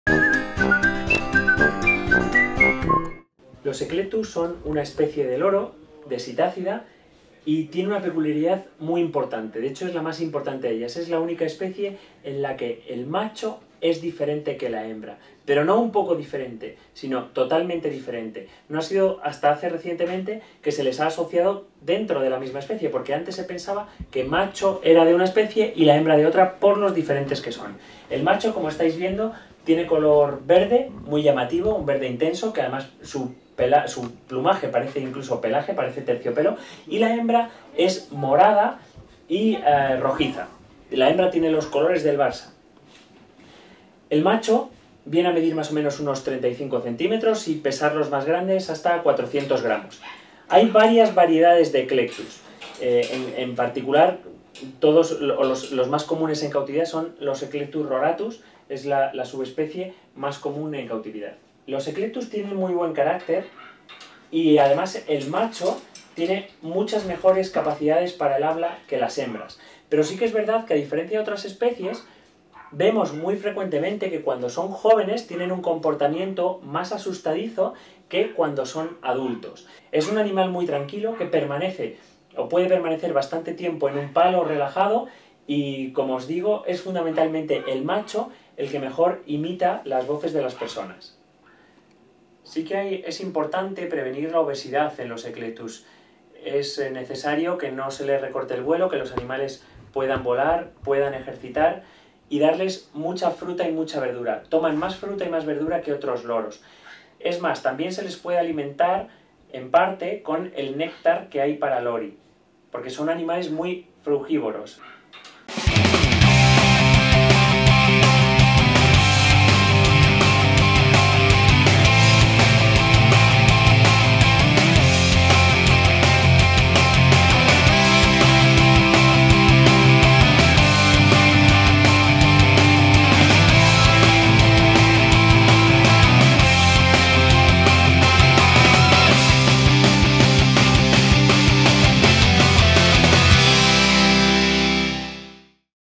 AVES - El macho de Eclectus, un loro espectacular
En este vídeo de EL CÓMO Y EL PORQUÉ DE MI MASCOTA os hablamos del macho de Eclectus, una citácida en la que el macho es completamente diferente a la hembra, lo que no suele suceder hasta este nivel. El macho es realmente espectacular en cuanto al plumaje. ¿Queréis verlo? ¡Dadle al play y sabréis mucho más de esta especie de loro.
Si quereis encontrar nuestros productos no dudeis en visitar nuestra web